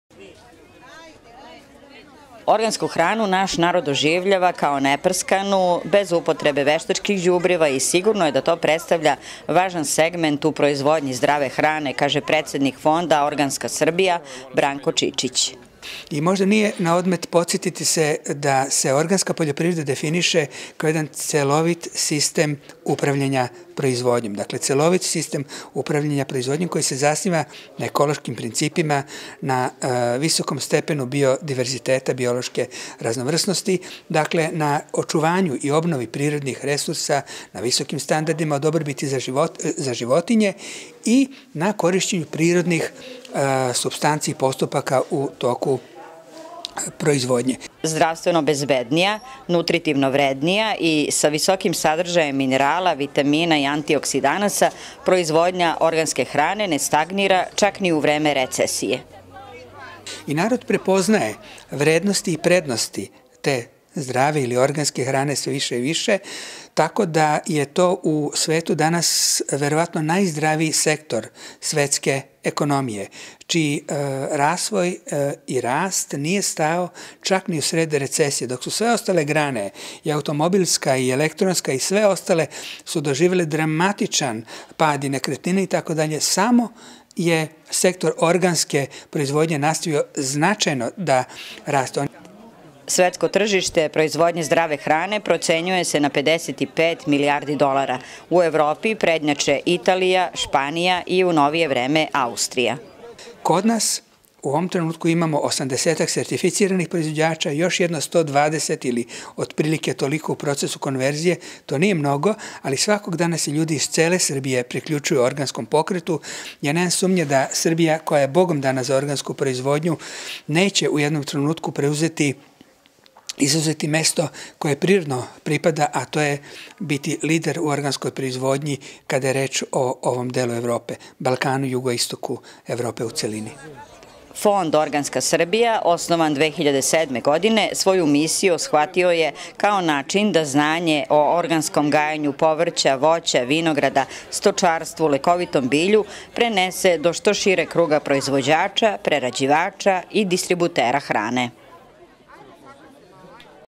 Zdrava hrana: prednost i šansa Srbije
Svetsko tržište proizvodnje zdrave hrane procenjuje se na 55 milijardi dolara.